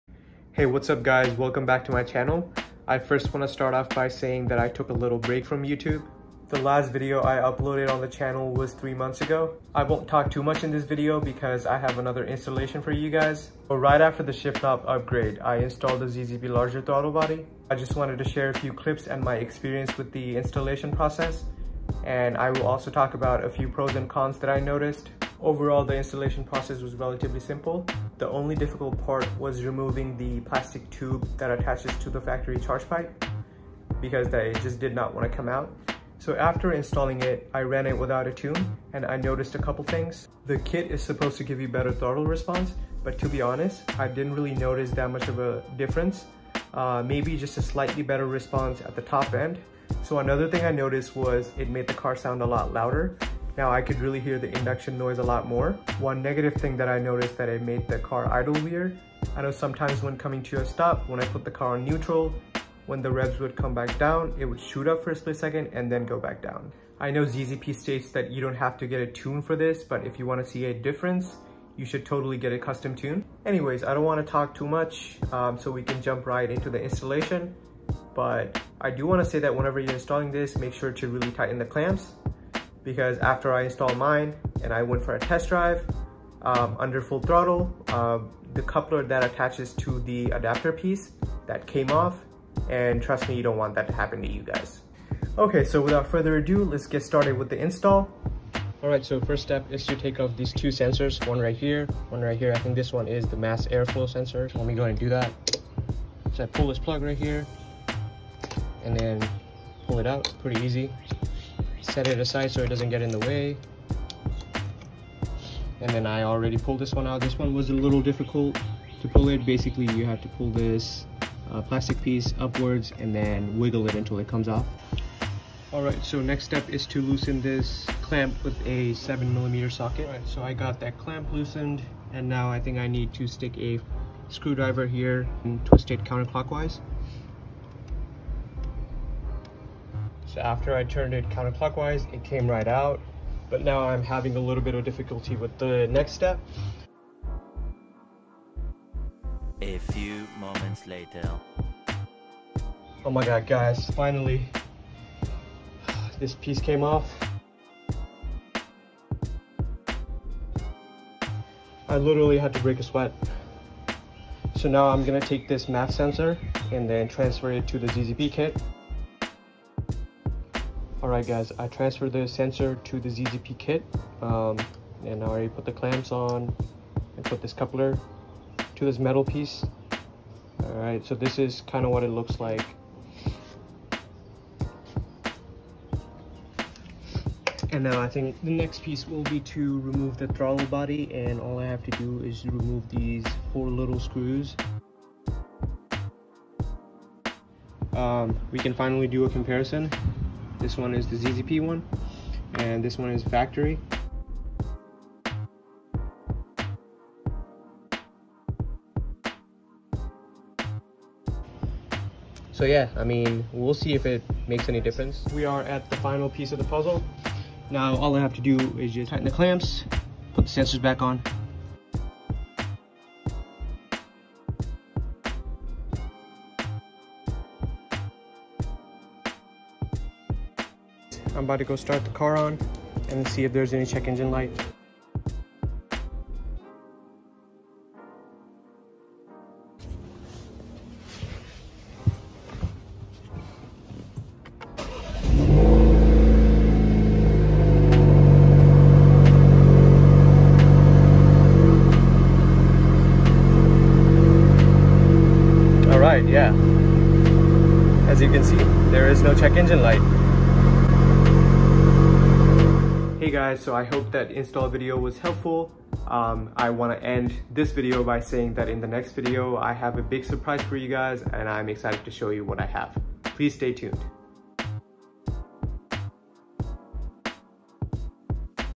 Installing The ZZP Larger Throttle Body - 2021 Camaro 2.0T
In this video, I share my experience with the installation process and overall thoughts on the ZZP Larger Throttle Body. Installation starts at 1:38